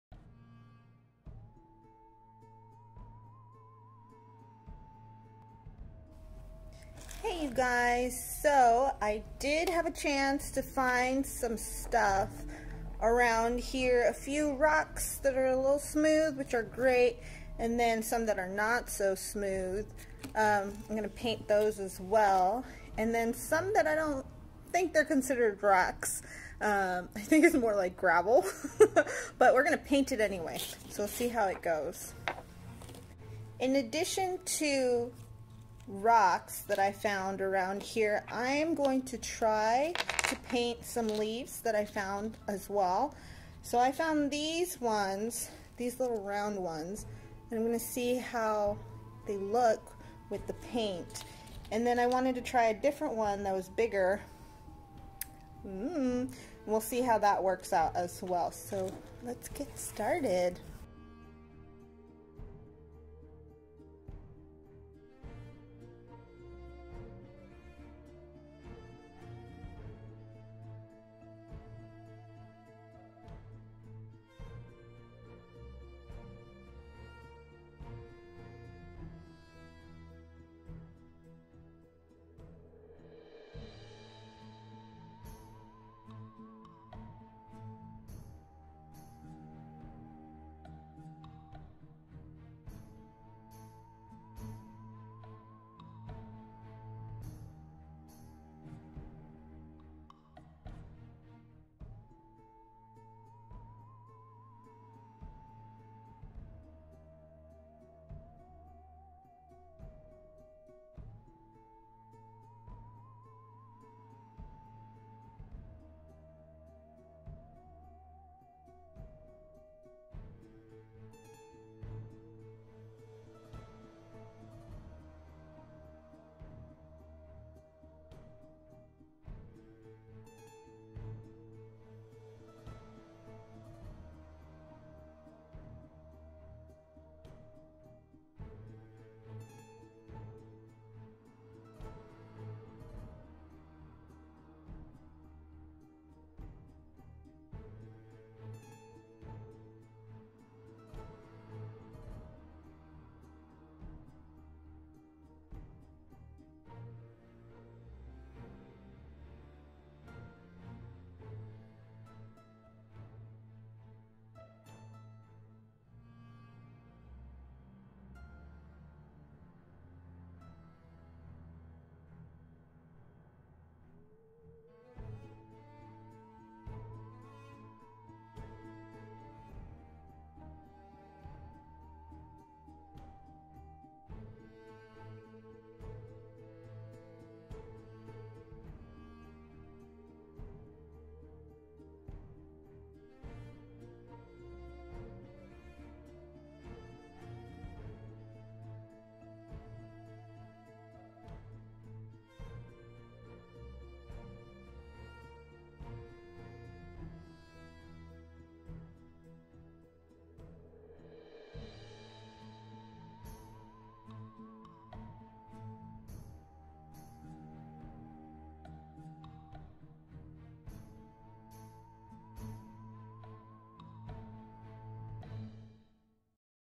Rock painting. Week 5
This week we are rock painting. You're welcome to use any types of rocks and/or leafs you find. When you are finished, make sure to give them to a friend or family member. See you Thursday!